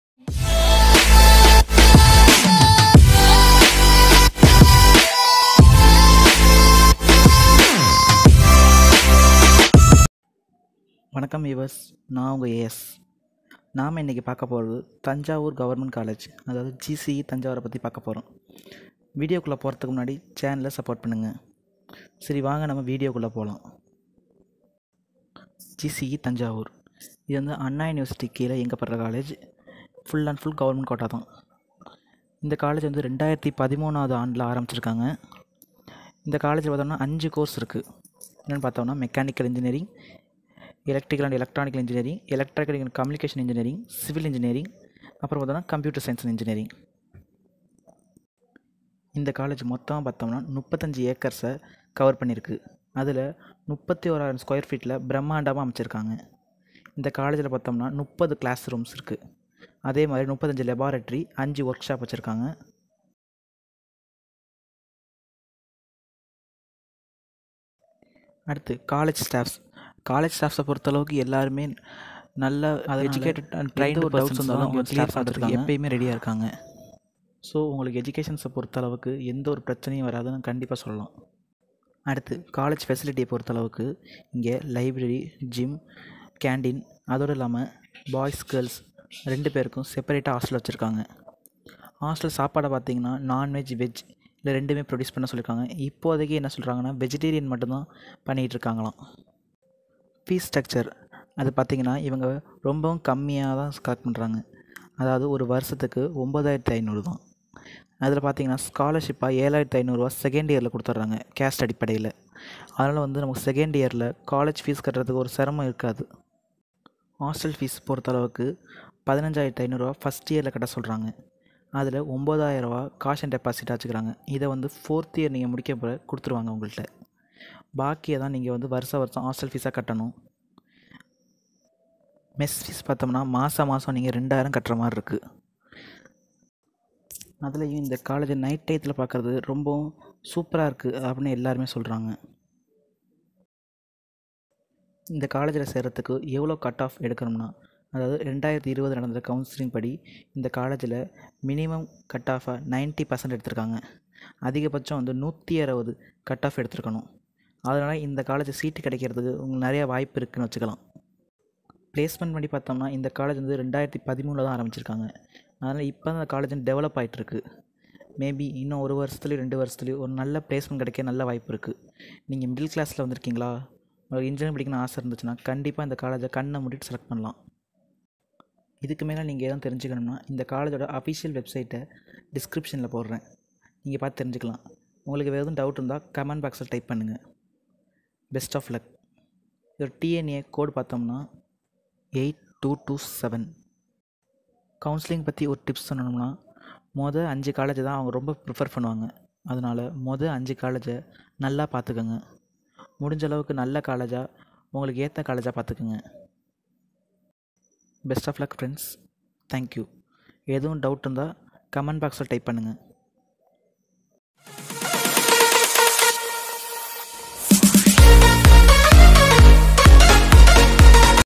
Government engineering college thanjavur 2021 full review (use headphone for better audio)
Government college of Engineering,sengitappati,gandharvakottai,
thanjavur
TNEA:8227
FEES STRUCTURE
STAFF
FACILITY
HOSTEL
PLACEMENT
CUTTOFF OF 2021